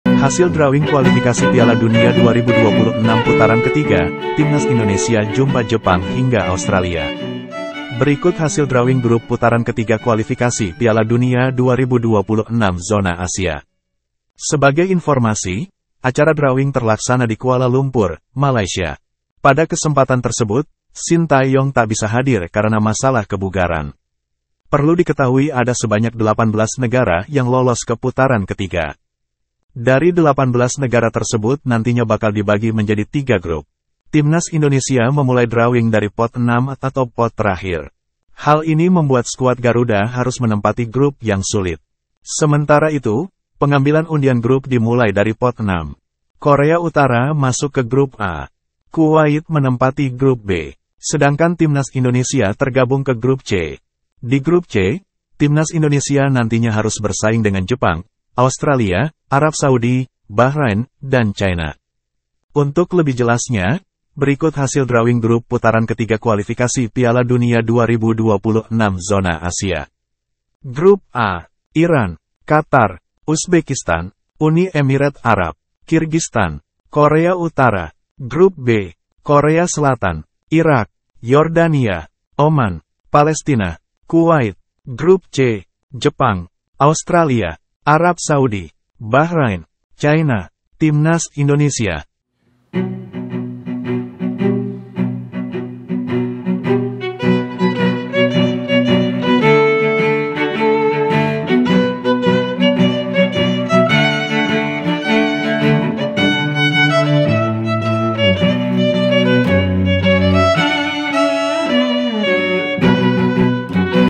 Timnas Indonesia Jumpa Jepang hingga Australia, Hasil Drawing Kualifikasi Piala Dunia 2026 Putaran 3
Grup A

Iran
Qatar
Uzbekistan
Uni Emirate Arab
Kirgistan
Korea Utara

Grup B

Korea Selatan
Irak
Yordania
Oman
Palestina
Kuwait

Grup C

Jepang
Australia
Arab Saudi
Bahrain
China
Timnas Indonesia

Editor Video : Prima Yudistira (PRX)